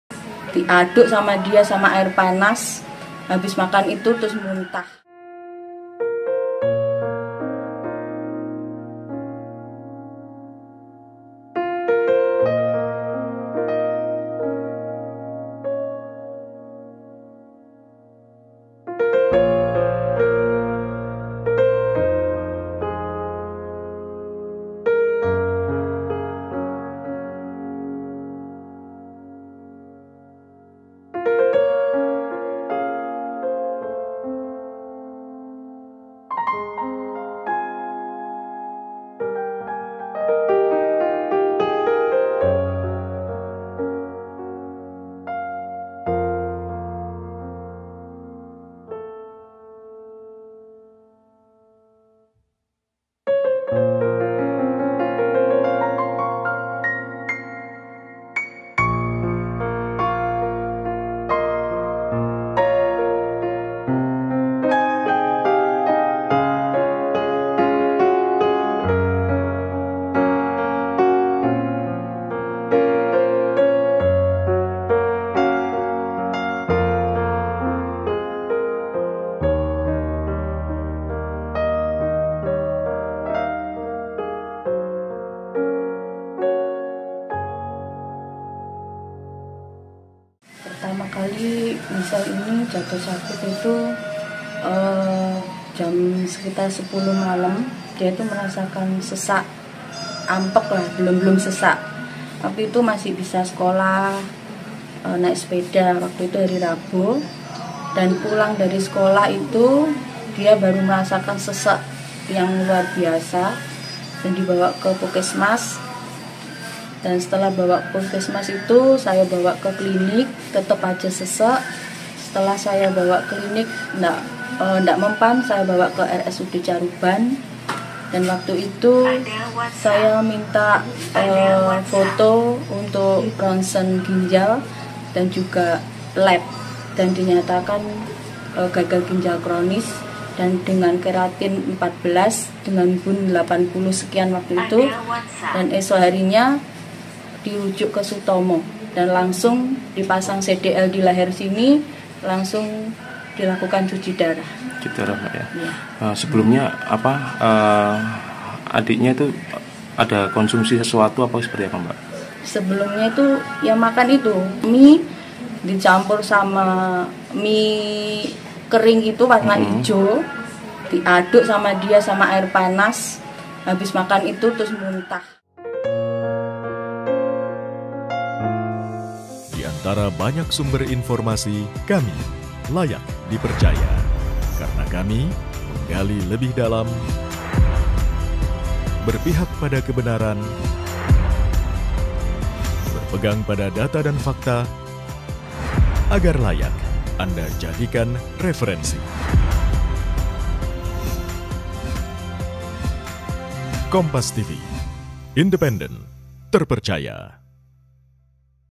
Seorang Anak di Surabaya Gagal Ginjal Akibat Gemar Konsumsi Makanan Instan
KOMPAS.TV - Seorang Anak di Surabaya Gagal Ginjal Akibat Gemar Konsumsi Makanan Instan